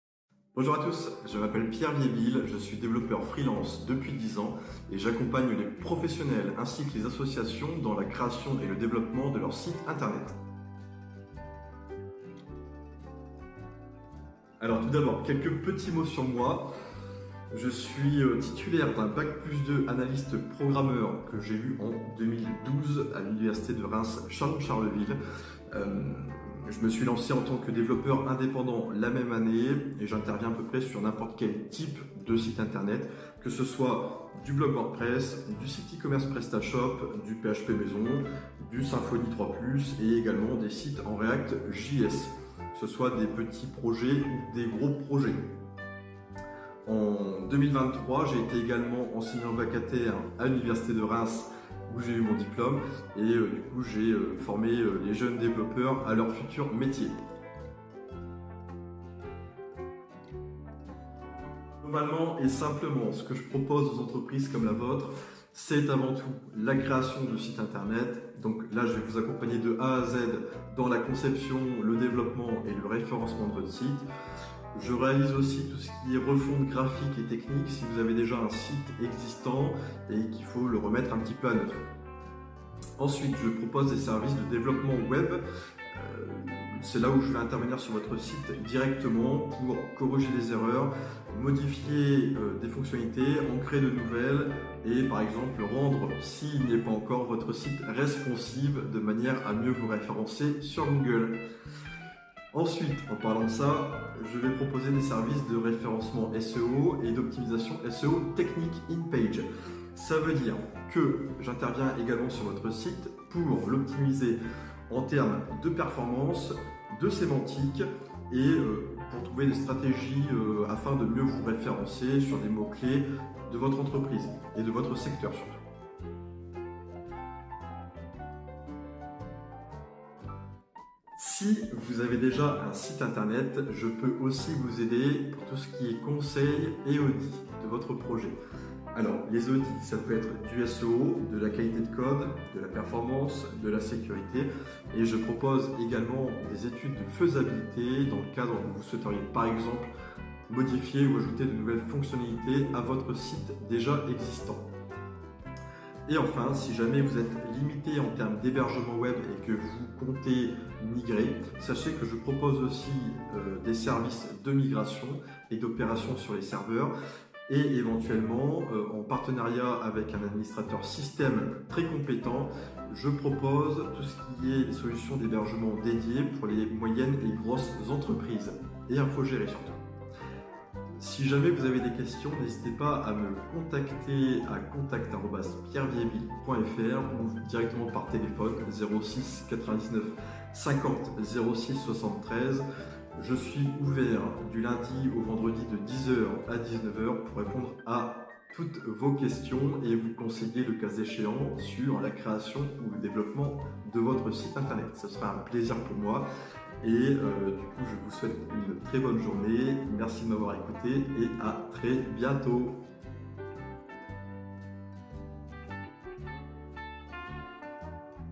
Présentation de Pierre Viéville: Développeur Web Freelance à Soissons.
Découvrez Pierre Viéville, développeur web indépendant à Soissons dans l'Aisne depuis 2012.

00:11 Formation et Parcours

Fort de plus de 12 ans d’expérience en développement web, j’ai acquis une solide expertise technique. Diplômé d’un DUT en Analyse-Programmation, j’ai su mettre mes connaissances à profit en lançant mon agence freelance dès 2012.

Mon parcours est marqué par une volonté continue d’apprentissage, notamment grâce à mon expérience en tant qu’intervenant vacataire à l’université, où j’ai pu partager mes connaissances sur des technologies de pointe comme Symfony et React.js.

 1:00 Agence Web Freelance

Basée à Soissons, mon agence web freelance intervient partout en France et à l’étranger. Je travaille à distance pour une clientèle variée, ce qui me permet d’être à votre écoute et de m’adapter à vos besoins spécifiques.

Maîtrisant les dernières technologies web, je propose des solutions performantes et sur-mesure, que vous soyez une TPE/PME, un organisme ou une agence web.

Mes contributions à des projets WordPress open-source utilisant l’Intelligence Artificielle (IA) comme Artist Image Generator participent également à ma visibilité.

Vous disposez d’une agence digitale à proximité.

03:00 Clients types

Je collabore avec des freelances, des artisans, des agences de communication, des commerces, des entreprises, des influenceurs et des associations.

Je suis également sollicité par des ESN et des agences web pour des missions ponctuelles.

Fort de 12 ans d’expérience, j’ai acquis une expertise dans de nombreux secteurs, tels que le commerce, le luxe, l’industrie, la musique, la finance, etc. Je travaille aussi avec des influenceurs, des Youtubeurs, des écrivains et des éditeurs de livres.

Plus qu’un simple prestataire, je suis un véritable partenaire digital. Je m’engage à vous accompagner tout au long de votre projet, de la conception à la réalisation, en passant par la maintenance et l’évolution.

Contactez-moi pour plus d’infos !

Pierre vous accompagne à toutes les étapes de votre site.